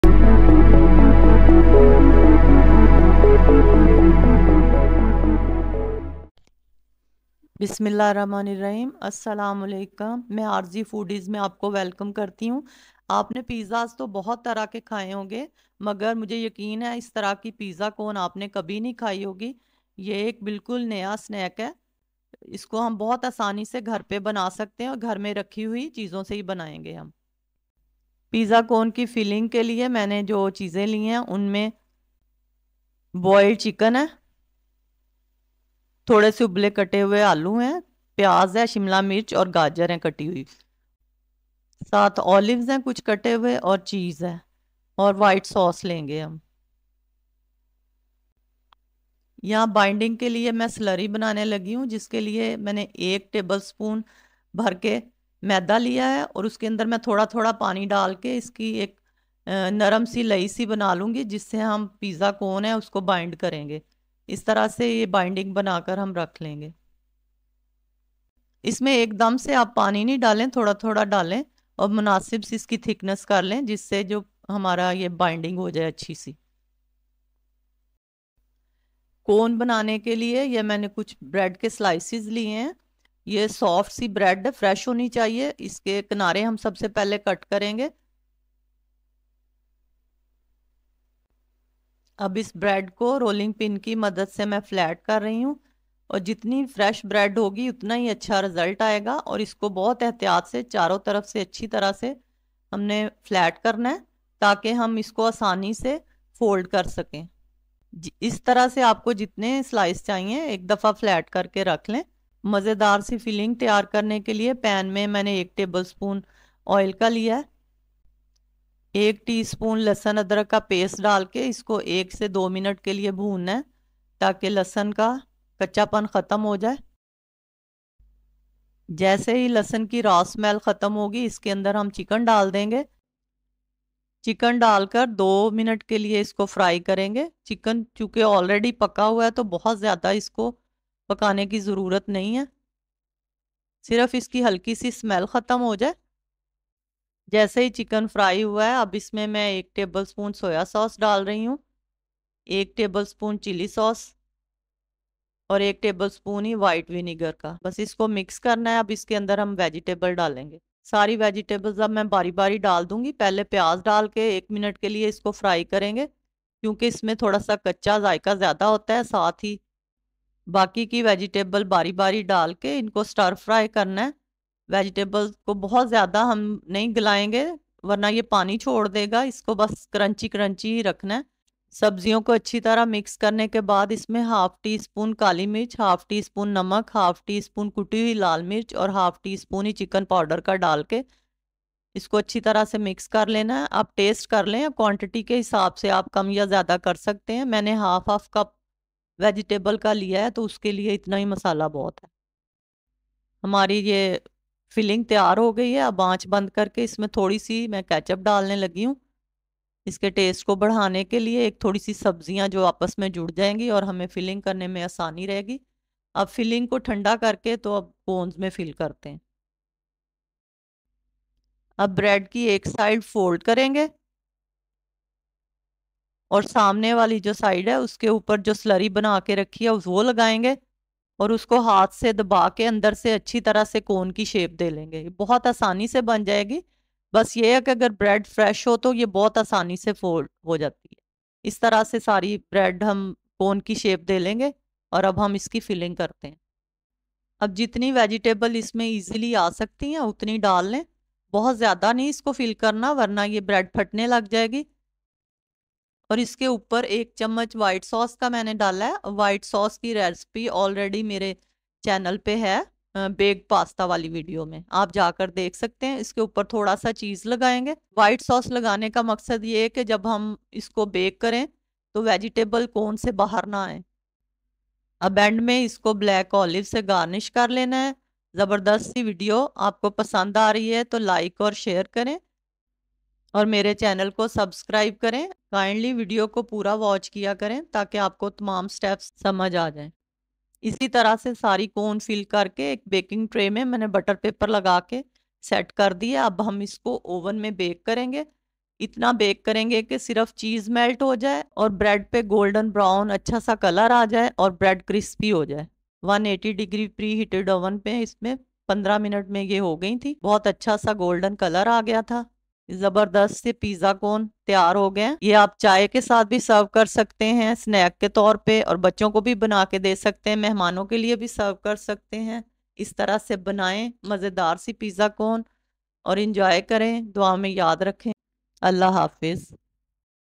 Pizza Cone Recipe by RZ Fudies || Quick & Easy Snack Recipe || How To Make Cheesy Pizza Cones
Bread (according to need)
Capsicums 1/2 cup
Boiled Potato 1/2cup
Boiled Chicken 1/2cup
Ketchup 2 tbsp
Flour Slurry 1 tbsp (for binding)
Salt halt tsp
Chicken Powder half tsp
Chilli Flakes Half tsp
Black  Pepper Half ts p

Welcome to RZ Fudies, where we're about to embark on a culinary adventure to create the most delicious and innovative snack - Cheesy Pizza Cones! If you're a pizza lover (who isn't?), get ready for a treat that combines the classic flavors of pizza in a fun and portable cone form. This recipe is not only quick and easy but also perfect for satisfying those cheesy cravings. Join us in the kitchen as we guide you through each step, ensuring you end up with the most mouthwatering pizza cones you've ever tasted.

In this video, we'll be exploring the art of transforming simple ingredients into a snack that's both visually appealing and incredibly tasty. From preparing the dough to crafting the perfect blend of cheeses and toppings, we'll leave no stone unturned. Our goal is to make the process accessible to everyone, whether you're a seasoned chef or a cooking novice. Get ready to impress your family and friends with this unique and delightful twist on traditional pizza.

But it's not just about the recipe; it's about the experience. We'll share some tips and tricks along the way to ensure your pizza cones turn out perfect every time. Whether you're hosting a movie night, a casual get-together, or just craving a savory snack, these pizza cones are versatile and sure to be a crowd-pleaser. So, grab your apron and let's dive into the world of cheesy goodness together!

The golden-brown crust, the ooey-gooey cheese, and the aroma of your favorite pizza toppings – it's a feast for the senses! We can't wait for you to join us on this culinary journey and try making these Cheesy Pizza Cones in your own kitchen. Let's get cooking and make some memories!